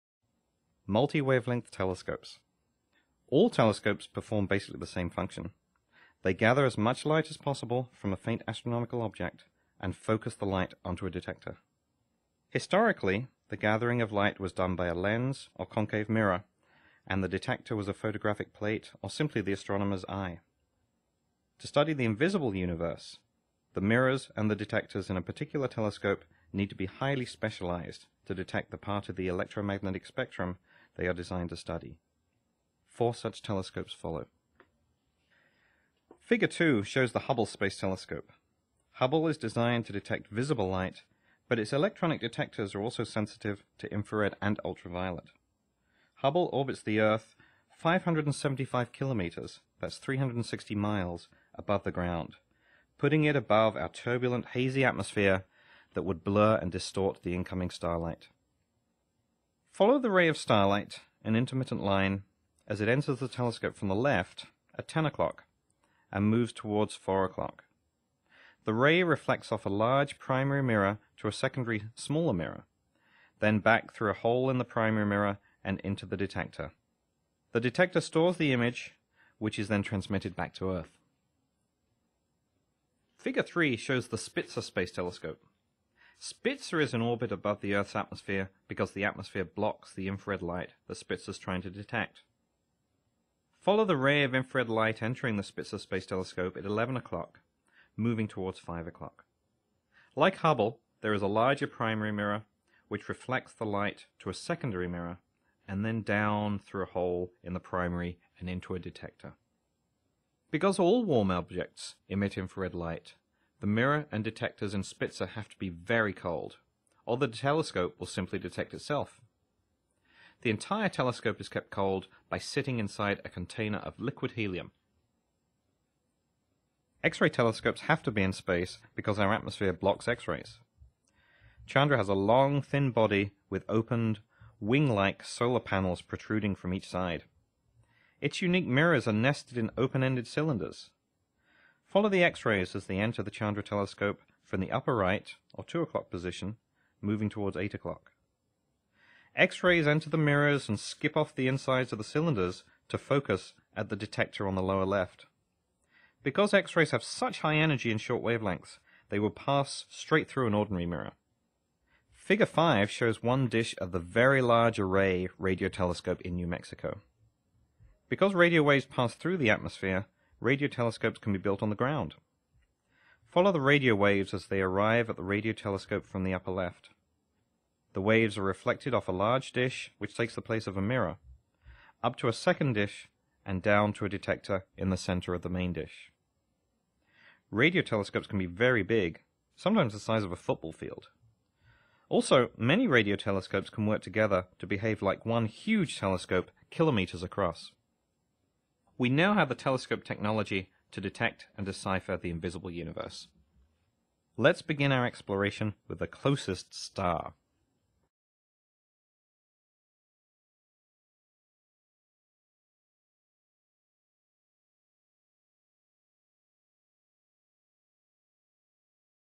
Chapter 2: Multi-Wavelength Telescopes (in AUDIO ONLY)
All telescopes perform basically the same function. They gather as much light as possible from a faint astronomical object, and focus the light onto a detector. Historically, the gathering of light was done by a lens or concave mirror, and the detector was a photographic plate, or simply the astronomer's eye. to study the invisible universe, the mirrors and the detectors in a particular telescope need to be highly specialized to detect the part of the electromagnetic spectrum they are designed to study. Four such telescopes follow.